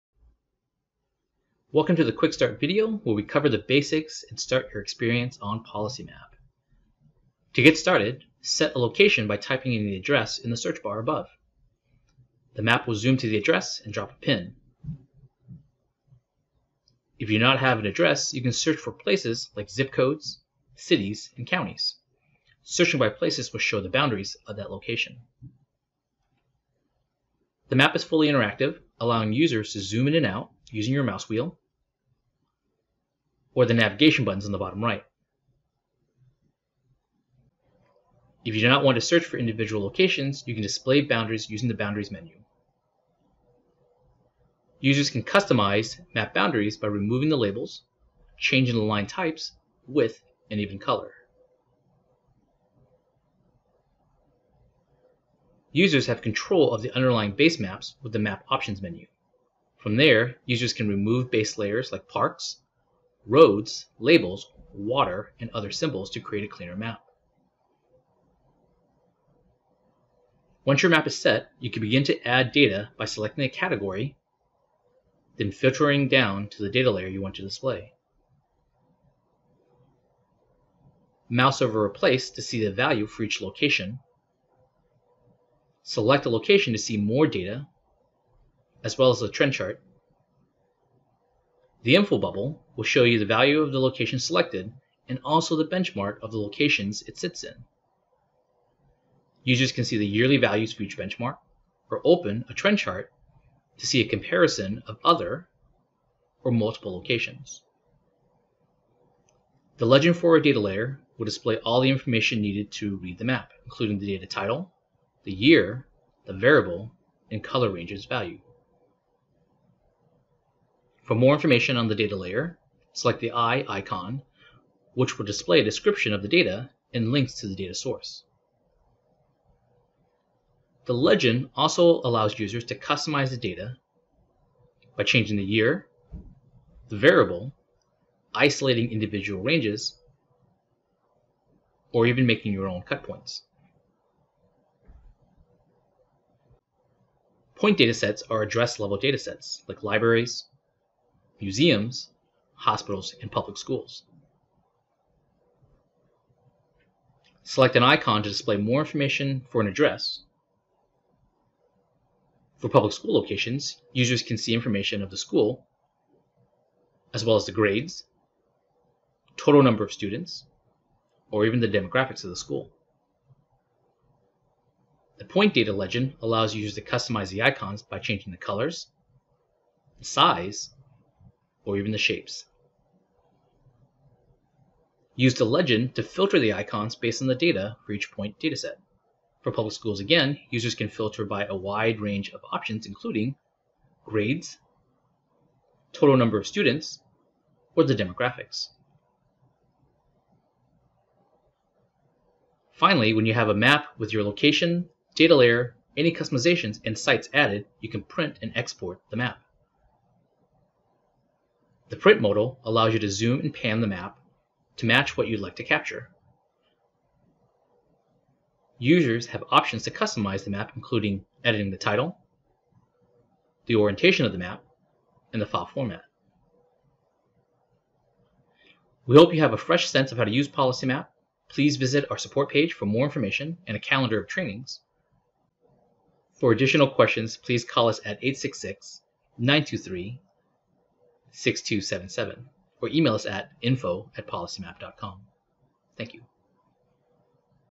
The New PolicyMap Quick Start
Jump right into making a map on the new PolicyMap. The quick start will walk users through how to make a map and also customizing data layers and points.